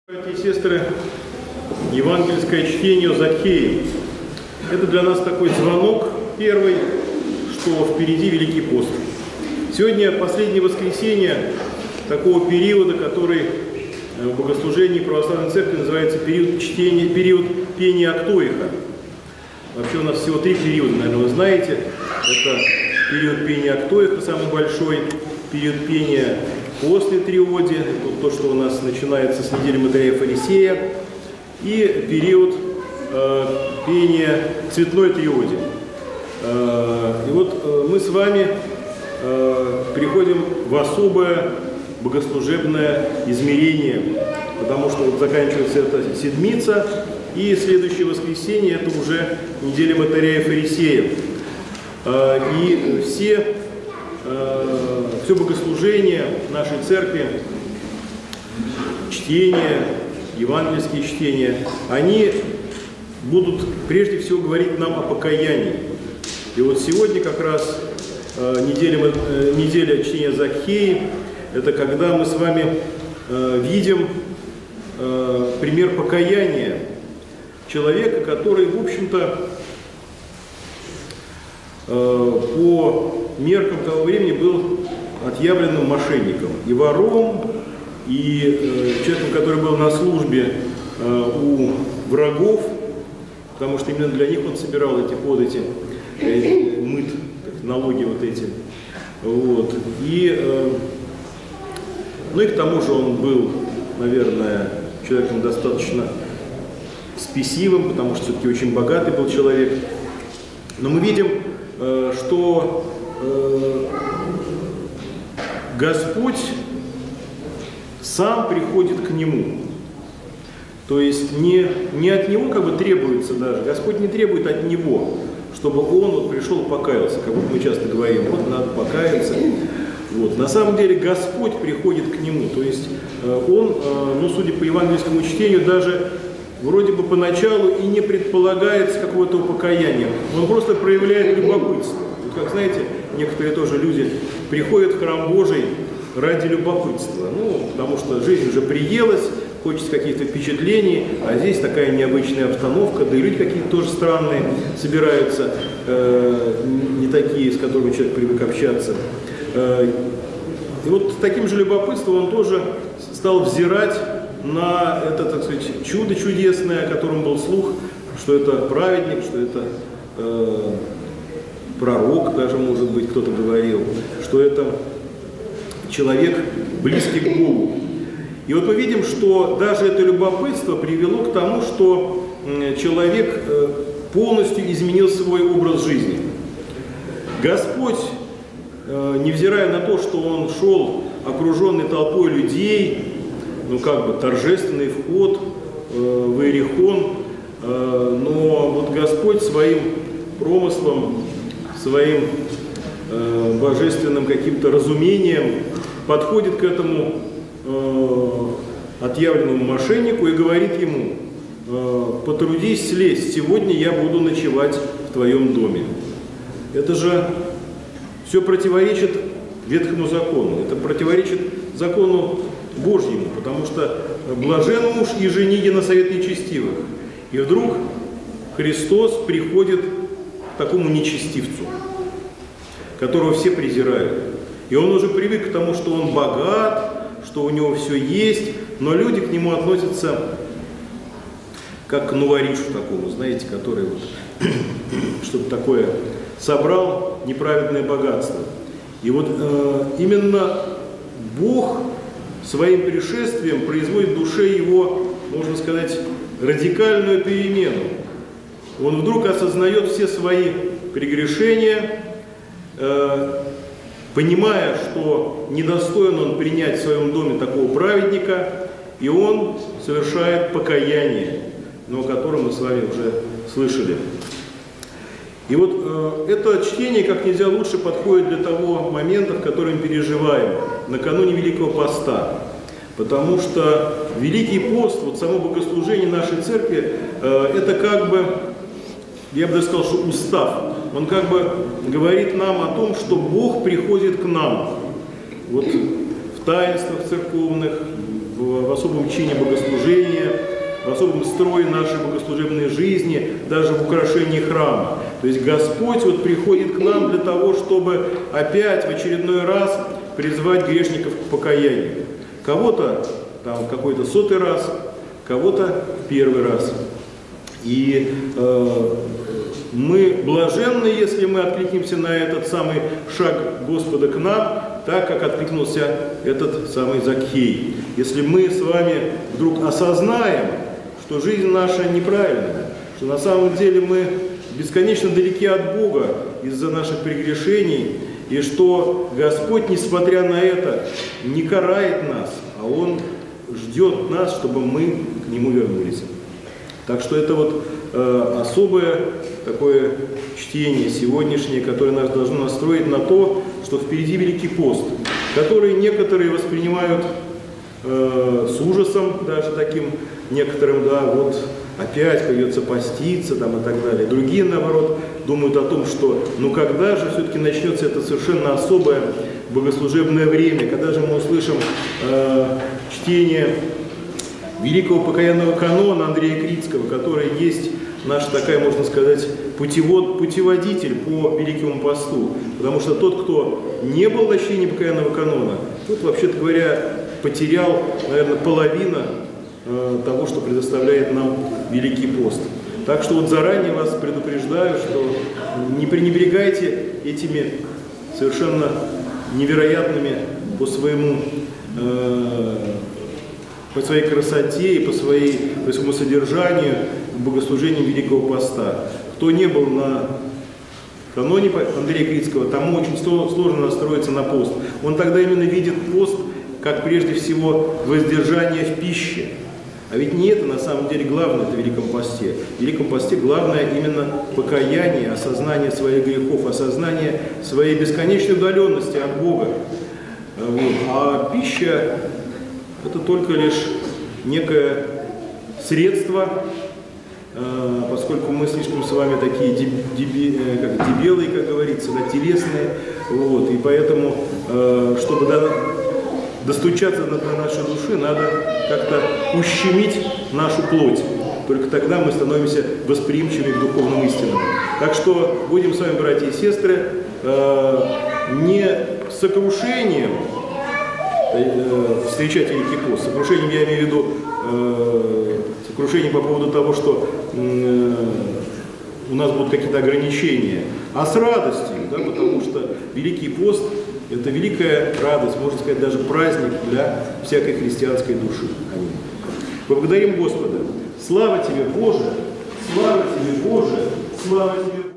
Отец Димитрий Проповедь 29 01 2023
Храм Преп. Феодора Студита у Никитских ворот.